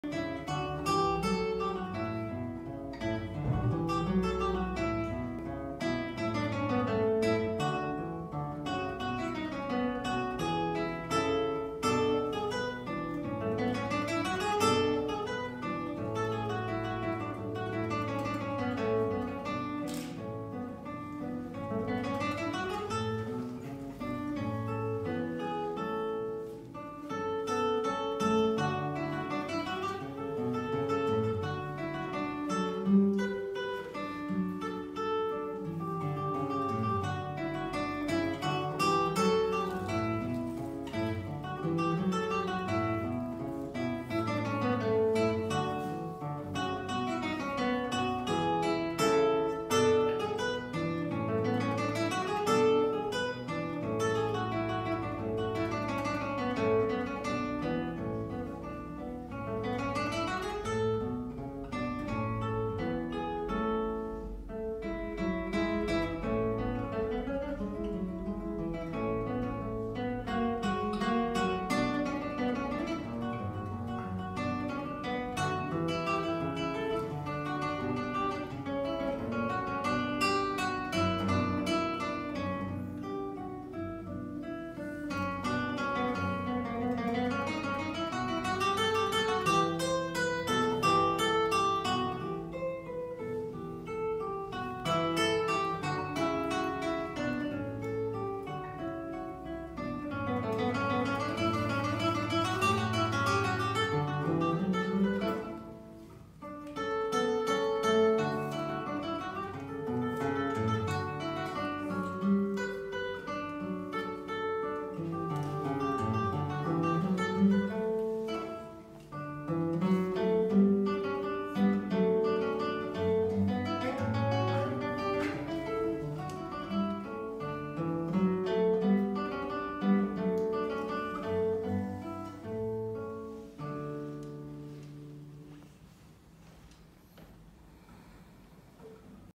Международный конкурс - фестиваль исполнителей на классической гитаре г. Нижний Новгород 2020 г.
23 февраля 2020 год